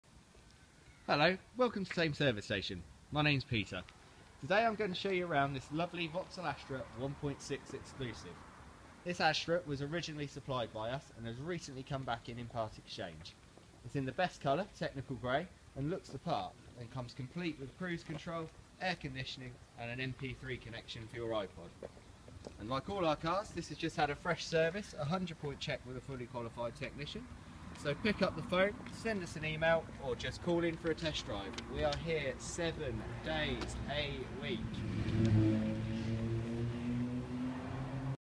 Thame Service Station Car Sales 2010 60 Vauxhall Astra 1.6 Exclusiv 5dr
Hello, welcome to Thame Service Station.
My name is James,
Today I'm going to show you around this lovely Vauxhall Astra 1.6 Exclusiv.
This Astra was originally supplied by us and has recently come back in part exchange. In the best colour, Technical grey it looks the part and comes complete with cruise control, air conditioning, and MP3 connection for your Ipod.
Like all our cars this has just had a fresh service, and a 100 point check with a fully qualified technician.